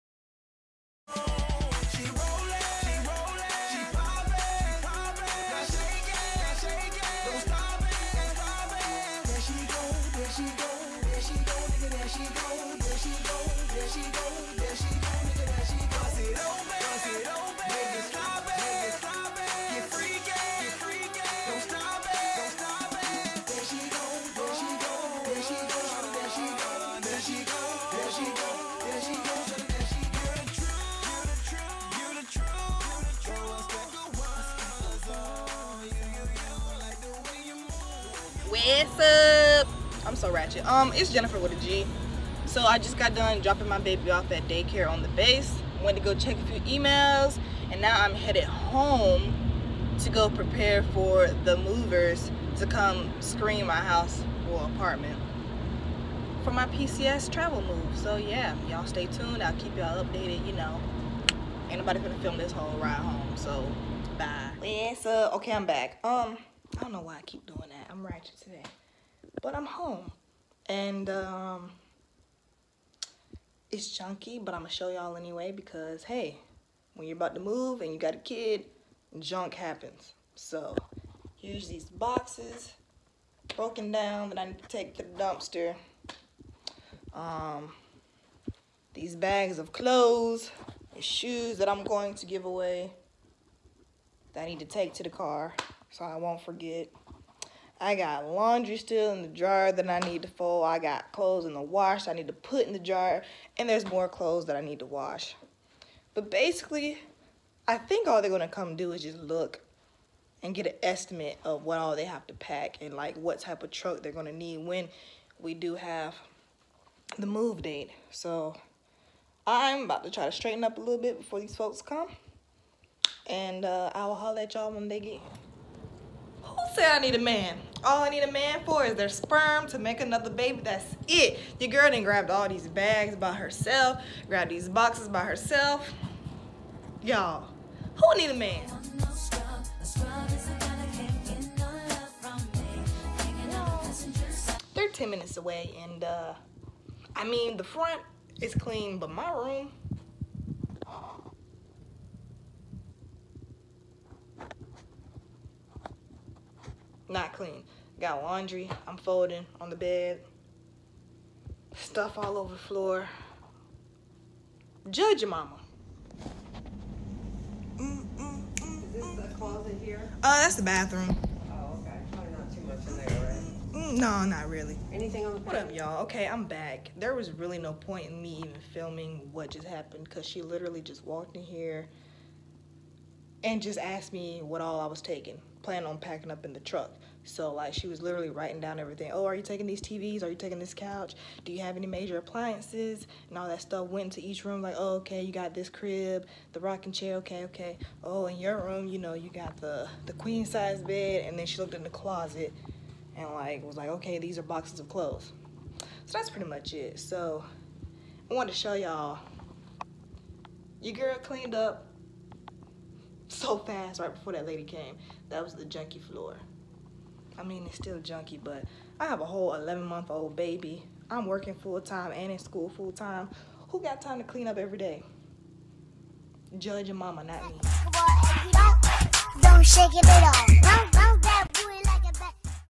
PCS Move Part 1
This is just a small snippet of what to look forward to when you’re about to PCS.